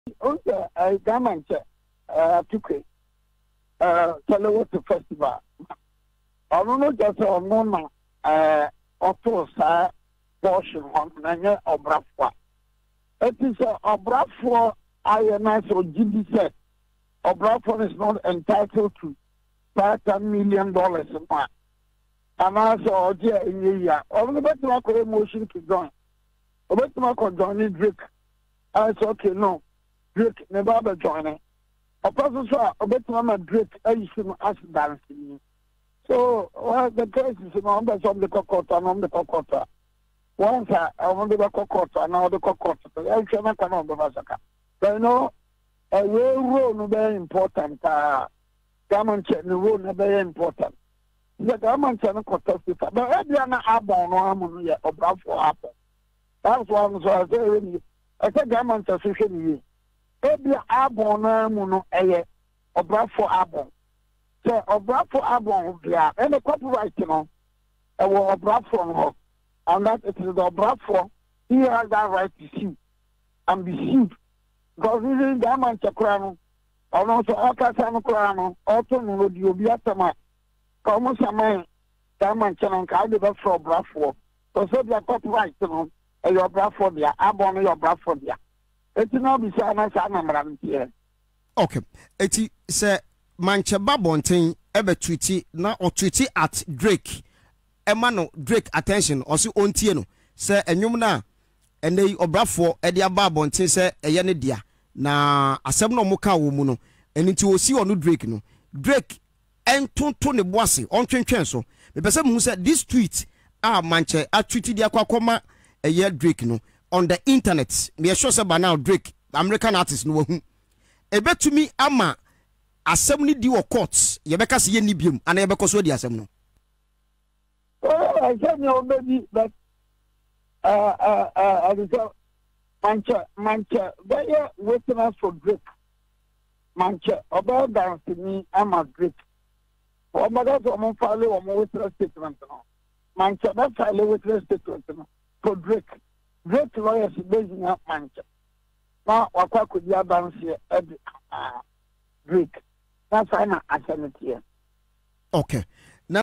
Obrafour - Drake lawsuit: Mantse does not own 'killer cut' sound if the album belongs to Obrafour
Obrafour - Drake lawsuit: Mantse does not own 'killer cut' sound if the album belongs to Obrafour -- Lawyer Obiri Yeboah